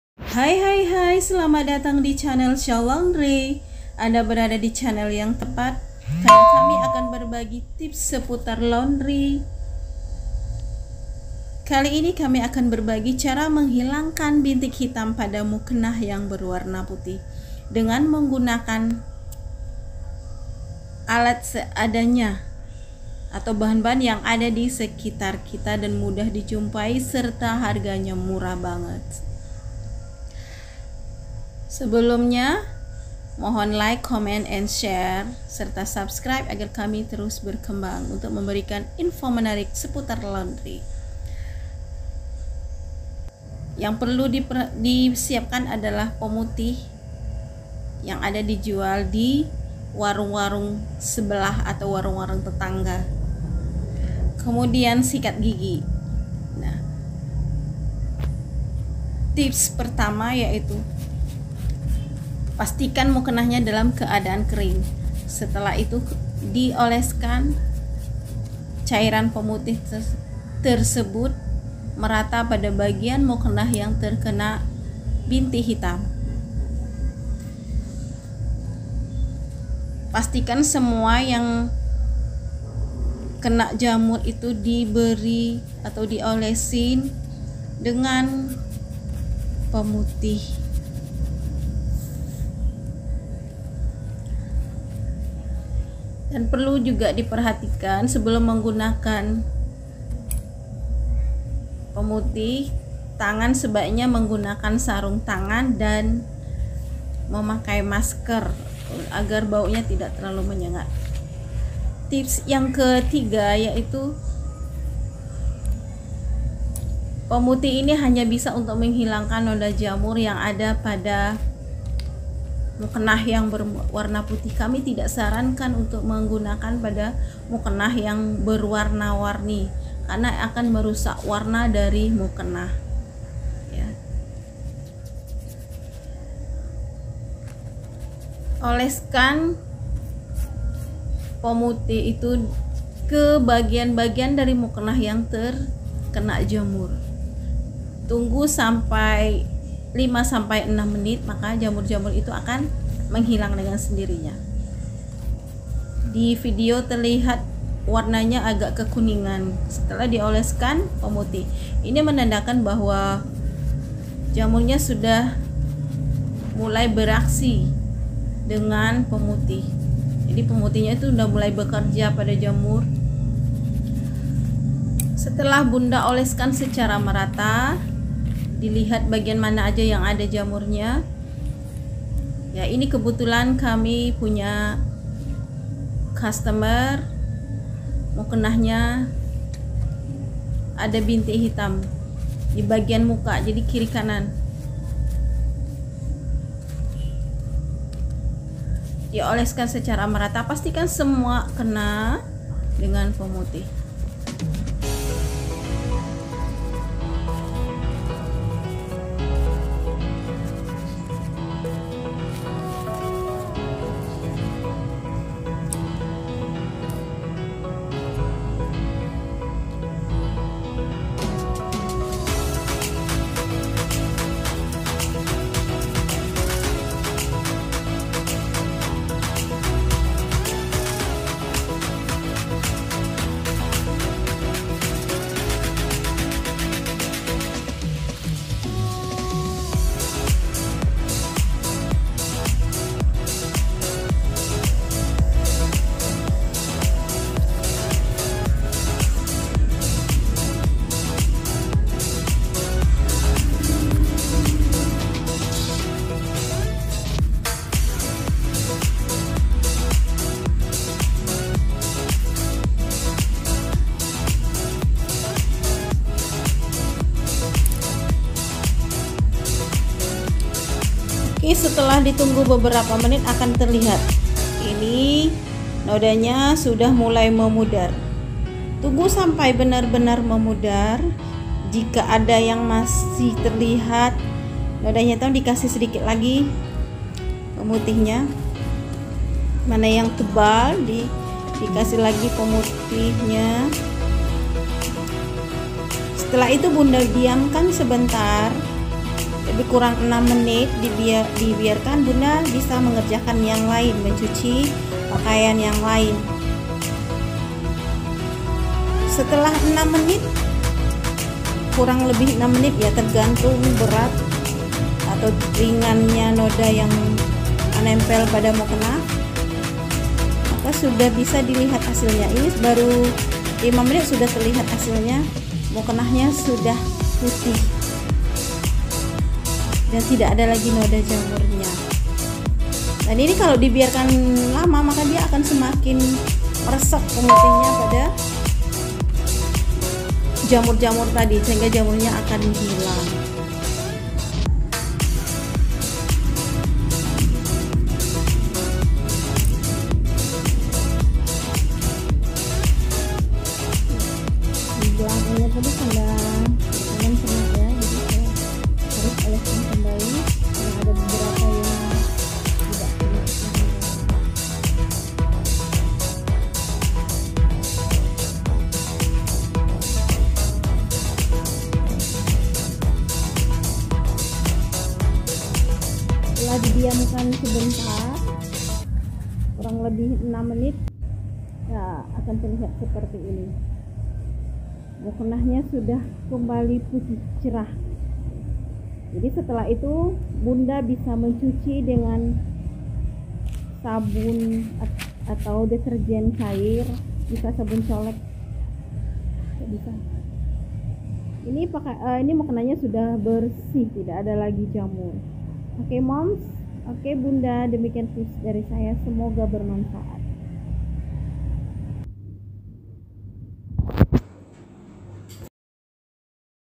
Cara membersihkan bintik hitam pada mukena
tahilalatmukenahilang bintik hitam atau jamur pada mukena sering banget di temui.
bintuk hitam ini muncul karena lembab.video ini berisi cara menghilangkan noda jamur / bintik hitam pada mukena dengan bahan yg mudah di dapat.